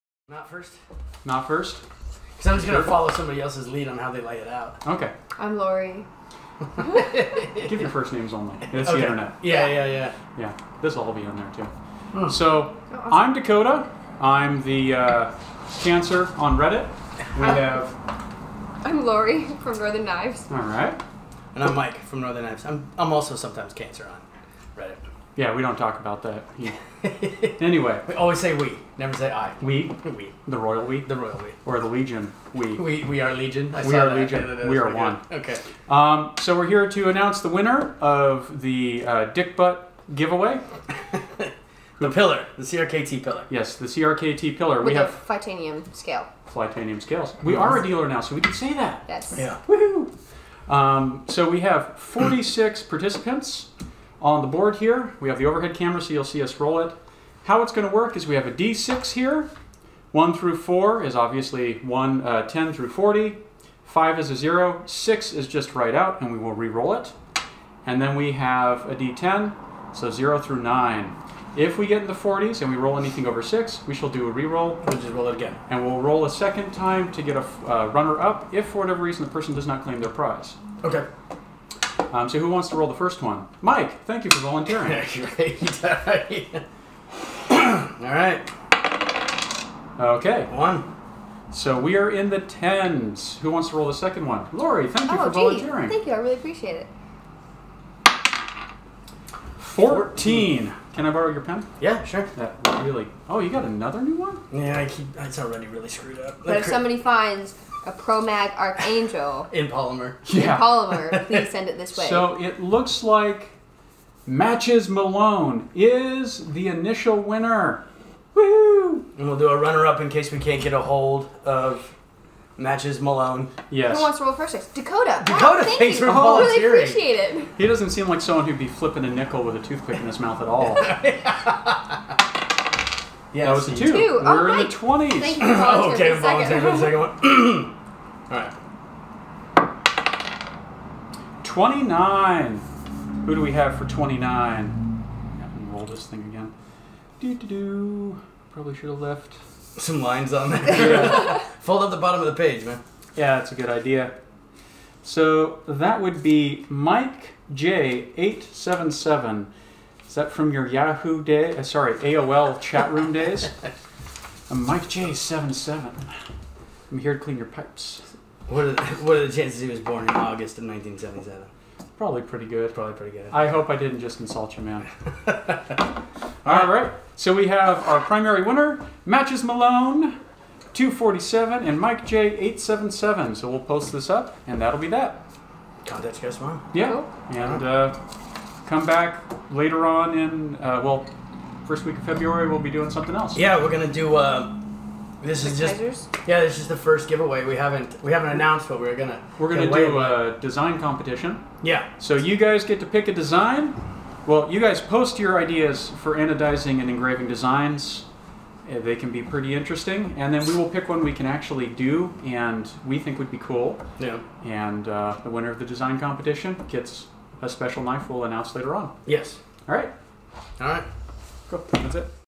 CRKT Pilar With Custom Anodized Titanium Scales Giveaway
This this short video we announce the winner of our January 2019 knife giveaway.

The winner of this competition is

Our runner up is

Or join us on YouTube every Monday for another installment on our YouTube channel.